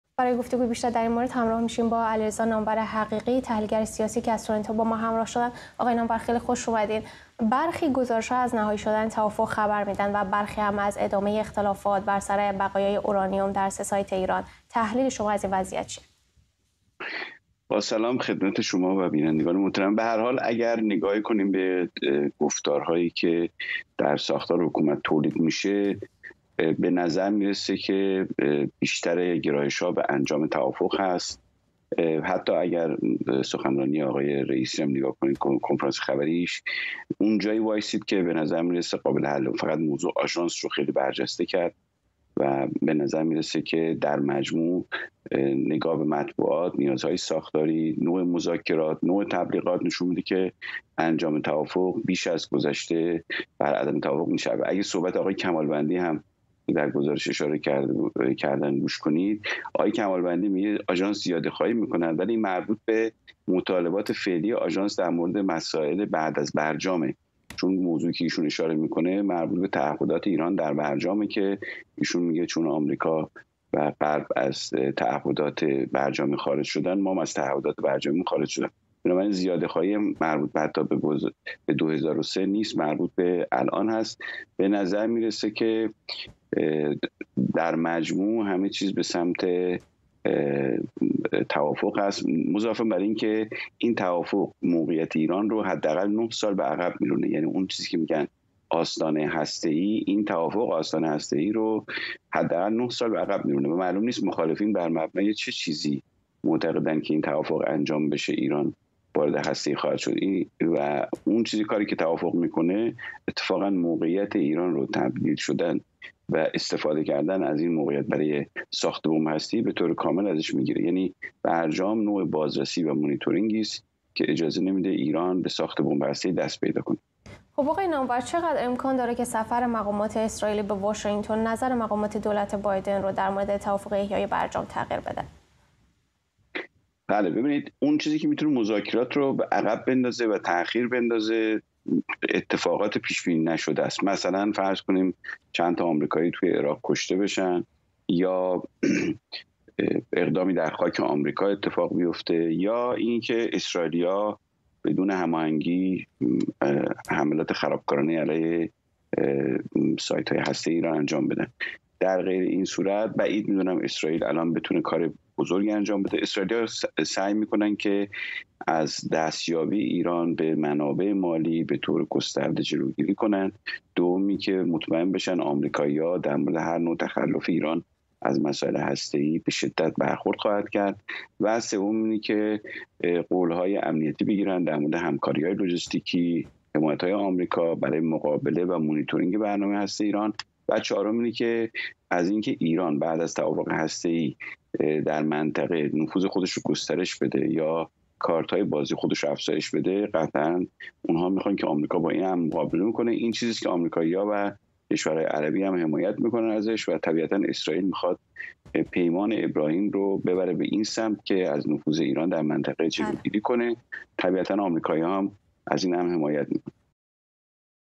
مذاکرات احیای برجام
علیرضا نامور حقیقی، تحلیلگر سیاسی: اسرائیل از آمریکا می‌خواهد که بعد از توافق هسته‌ای با جمهوری اسلامی در صورت اقدام به گسترش نفود خود در منطقه یا افزایش کارت‌های بازی‌اش با او مقابله کند